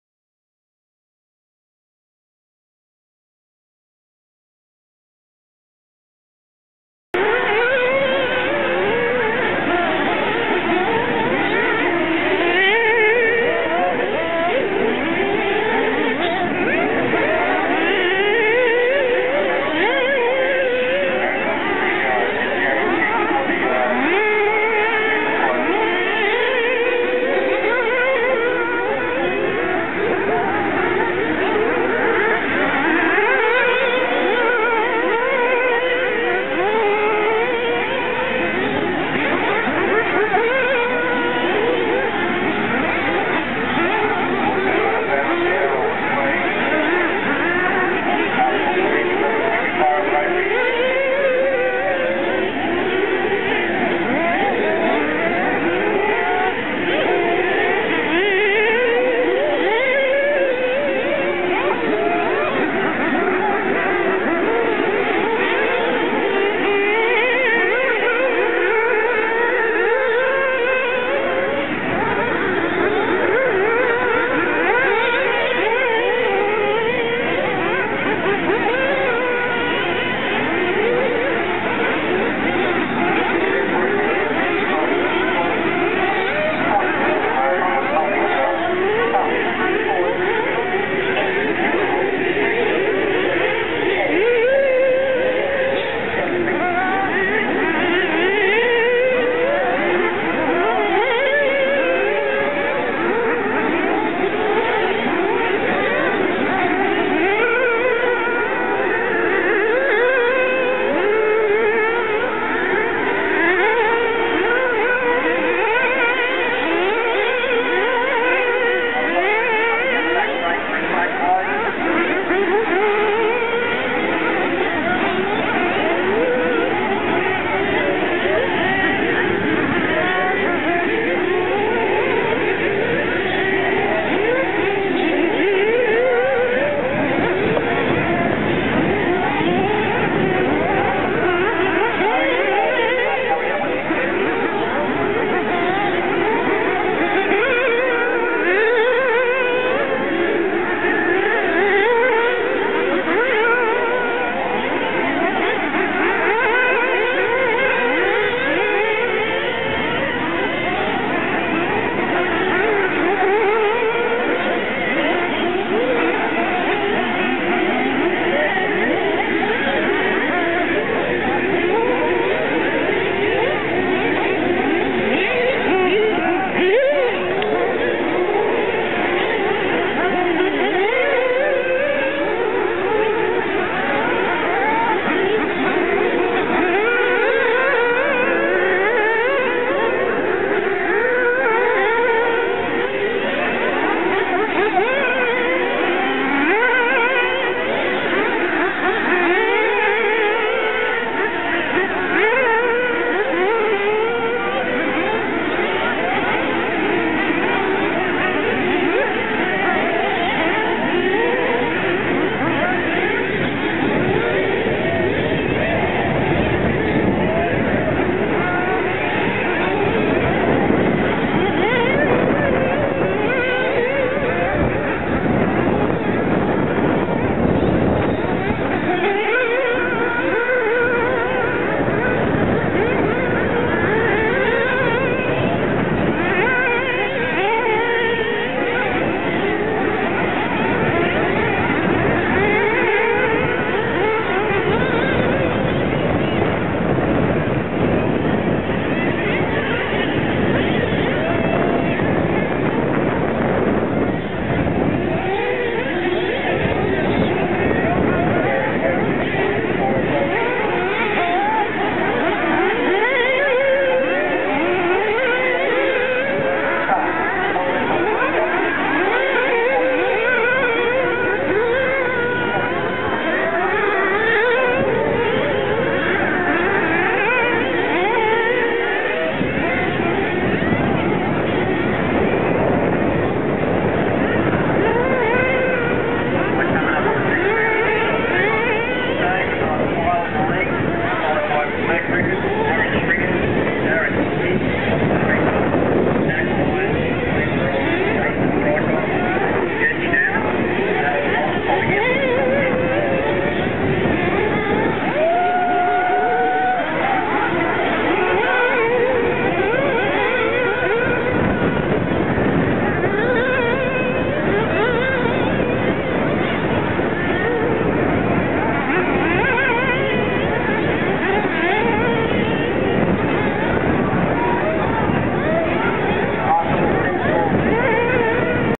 Wot Hobbies Aaron Stringer
Wot Hobbies & Melbourne Off Road Radio Car Club present the Australian Buggy & Truggy Nationals in Sunshine, Victoria, Australia, here is some footage during the A main final of the buggy action